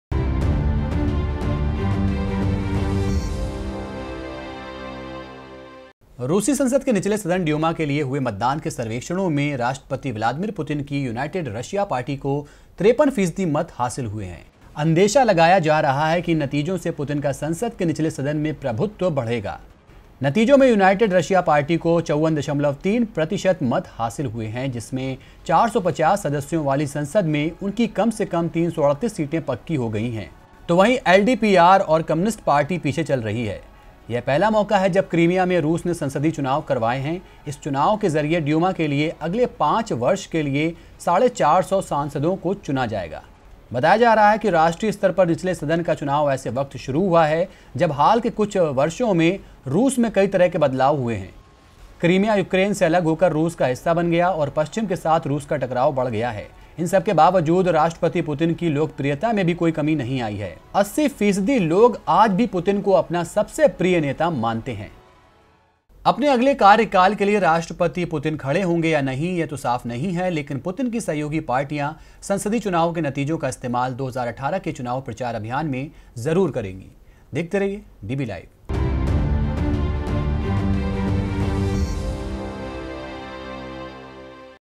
DB LIVE | 19 SEPTEMBER 2016 | Big victory for Putin-backed party United Russia | रूस में चुनाव
रूसी संसद के निचले सदन ड्यूमा के लिए हुए मतदान के सर्वेक्षणों में राष्ट्रपति व्लादिमीर पुतिन की यूनाइटेड रशिया पार्टी को 53 फीसदी मत हासिल हुए हैं। अंदेशा लगाया जा रहा है इन नतीज़ो से पुतिन का संसद के निचले सदन में प्रभुत्व बढ़ेगा..
...नतीज़ों में यूनाइटेड रशिया पार्टी को 54.3 प्रतिशत मत हासिल हुए हैं, जिससे 450 सदस्यों वाली संसद में उनकी कम से कम 338 सीटें पक्की हो गई हैं। तो वहीं एलडीपीआर और कम्युनिस्ट पार्टी पीछे चल रही है। यह पहला मौका है जब क्रीमिया में रूस ने संसदीय चुनाव कराए हैं। इस चुनाव के ज़रिए ड्यूमा के लिए अगले पांच वर्ष के लिए 450 सांसदों को चुना जाएगा। बताया जा रहा है कि राष्ट्रीय स्तर पर निचले सदन का चुनाव ऐसे वक्त शुरु हुआ है जब हाल के कुछ बरसों में रूस में कई तरह के बदलाव हुए हैं, क्रीमिया यूक्रेन से अलग होकर रूस का हिस्सा बन गया और पश्चिम के साथ रूस का टकराव बढ़ गया है। इन सबके बावजूद राष्ट्रपति पुतिन की लोकप्रियता में कोई कमी नहीं आई है। 80 फीसदी लोग आज भी पुतिन को अपना सबसे प्रिय नेता मानते हैं।